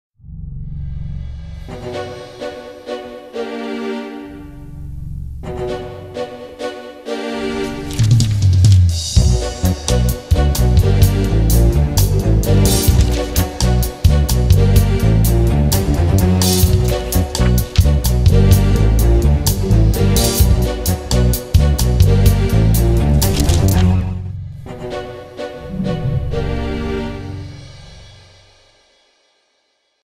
Bajka nad bajkama - Senka u tami
Trejler za knjigu "Bajka nad bajkama: Senka u tami", Nenad Gajić izdavač: Laguna 2013.

Muzika: Talisman
Montaža i režija: Bogdan Čurguz